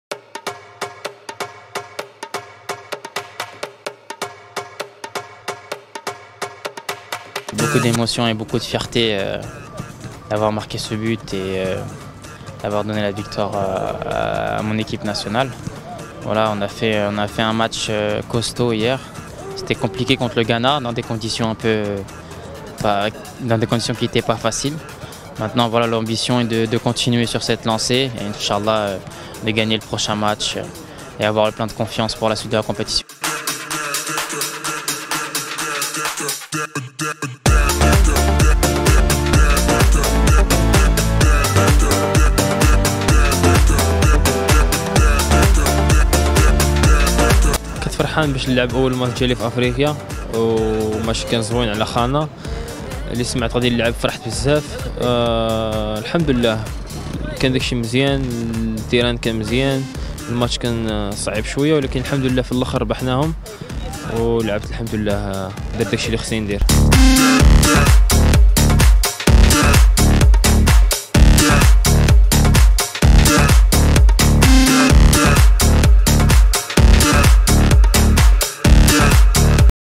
Sofiane Boufal أسود الأطلس تواصل إستعداداتها للقاء جزر القمر
إستعدادا للمباراة التي ستجمعه يوم الجمعة 14 يناير 2022 ابتداء من الساعة الخامسة مساء بمنتخب جزر القمر برسم الجولة الثانية عن المجموعة الثالثة من دور المجموعات لكأس إفريقيا للأمم الكاميرون 2021، يواصل المنتخب الوطني المغربي تحضيراته لهذه المباراة والتي تسعى العناصر الوطنية خلالها إلى حسم التأهل إلى الدور الثاني من هذه البطولة القارية.

وأجرى المنتخب الوطني المغربي حصة تدريبية مسائية يومه الأربعاء 12 يناير 2022 إنطلقت على الساعة الخامسة مساء وإمتدت لساعة ونصف، كما شملت تمارين بدنية وتقنية فيها الفردية منها والجماعية، كما عرفت هذه الحصة التدريبية مشاركة جميع العناصر بإستثناء اللاعبين رايان مايي وأيمن برقوق.